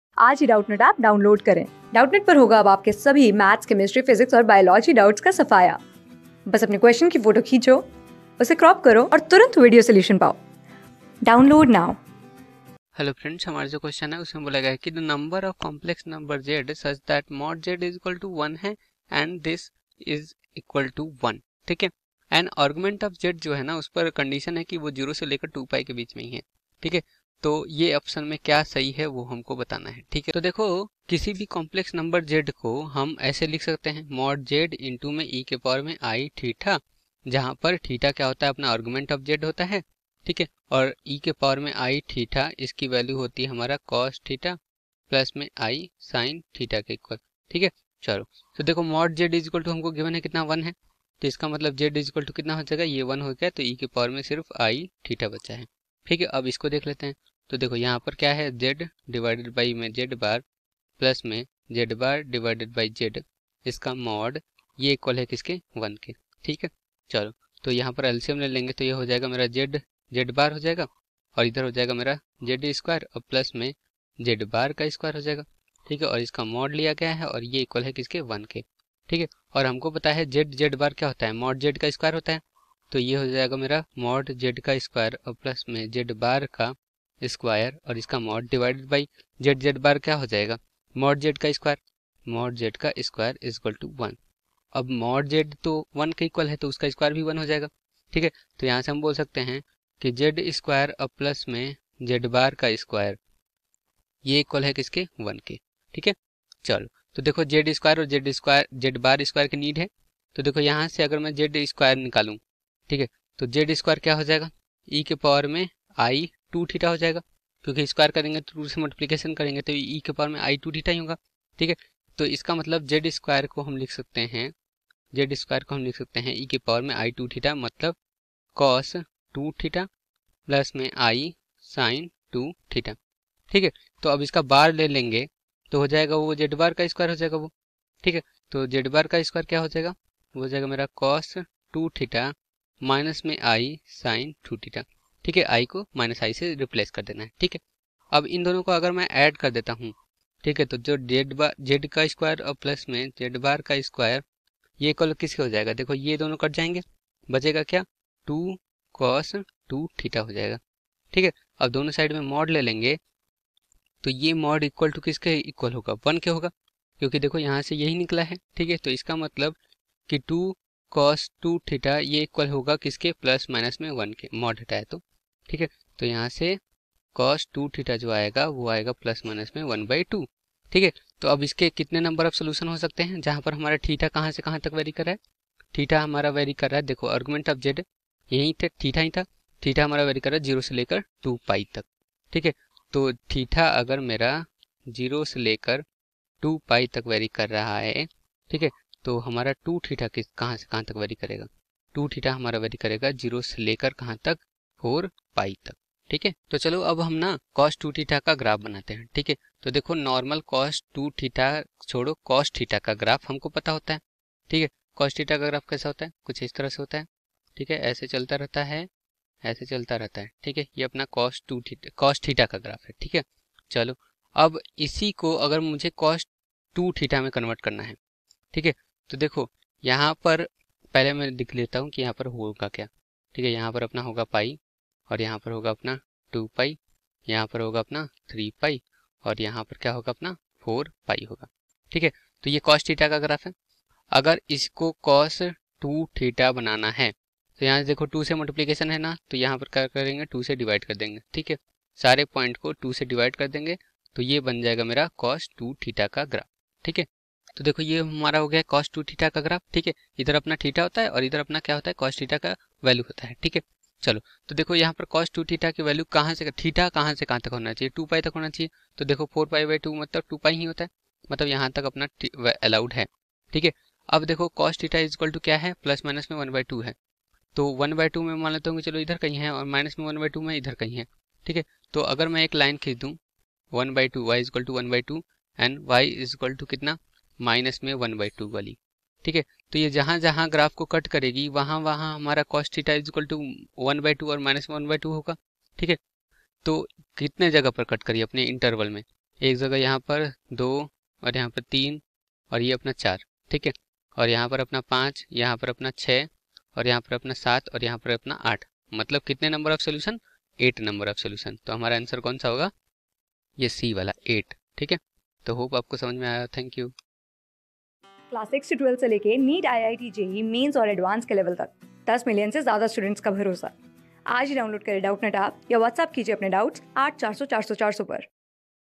The number of complex numbers z\nsuch that|=1 and |z/ barz +barz/z|=1\nis arg(z) in [0,2pi)) ...
The number of complex numbers z\nsuch that |z|=1 and |z/ barz +barz/z|=1\nis arg(z) in [0,2pi)) then \n\na. 4\nb. 6\nc. 8\nd. more than 8\n
Class: 11
Subject: MATHS
Chapter: COMPLEX NUMBERS AND QUADRATIC EQUATIONS
Board:IIT JEE

👉 Have Any Query? Ask Us.